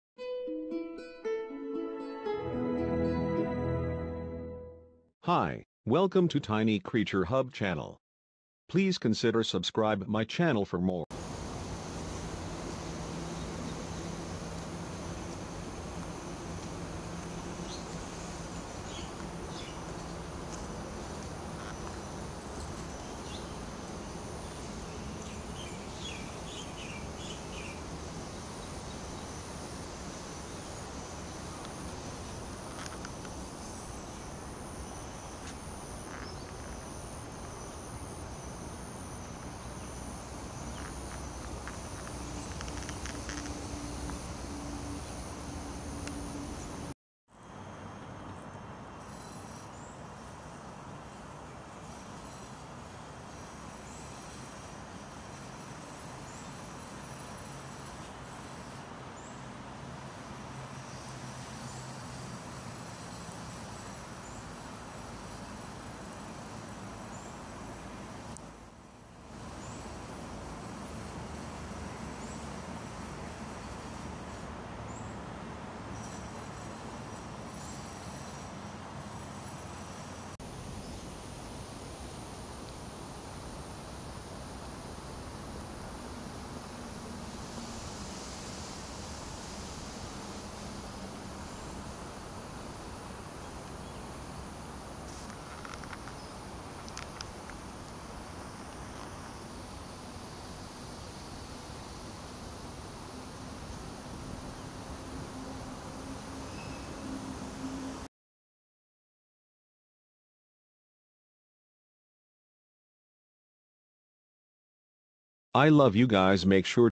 Singapore Bugs Two Tail Spider Camoflage Blend in trees insect macro video
Very common spider you can find in Singapore only if you are aware of it. Cause it camoflage is good. You may not see it right away🙊 In resting position, the flattened lichen-coloured body is pressed against the tree trunk, thus blending perfectly with the surrounding bark,
The only Singaporean who record tiny insect video in YouTube using a smartphone